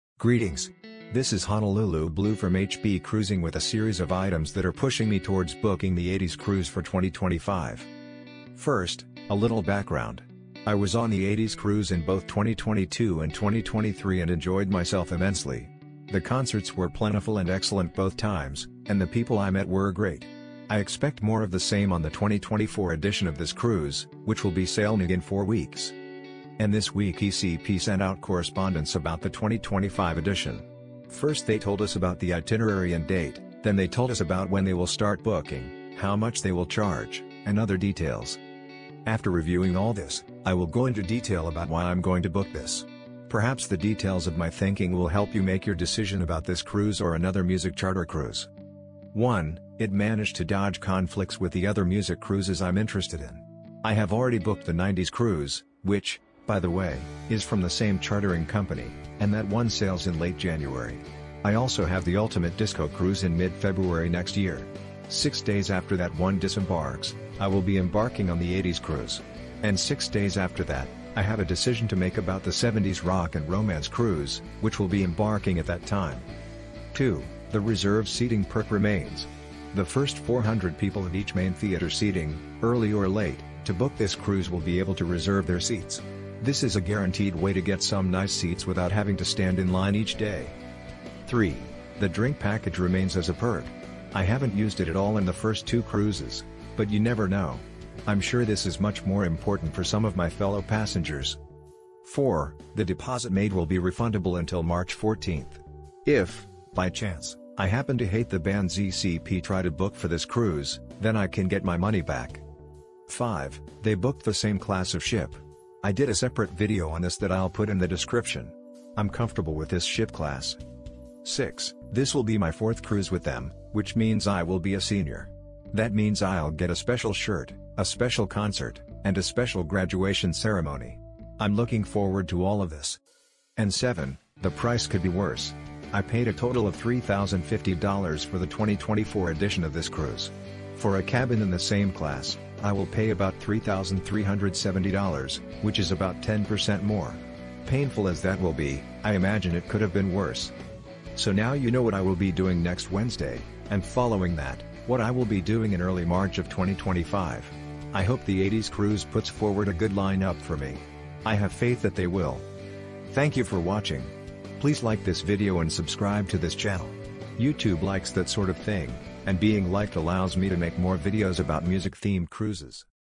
Why The 80s Cruise is RIGHT FOR ME in 2025
In this video I go into some detail about the correspondence I have received from Entertainment Cruise Productions (ECP) about the 2025 edition of The 80s Cruise.  Why do I feel the need to book it as soon as it comes available?  Watch the video to find out!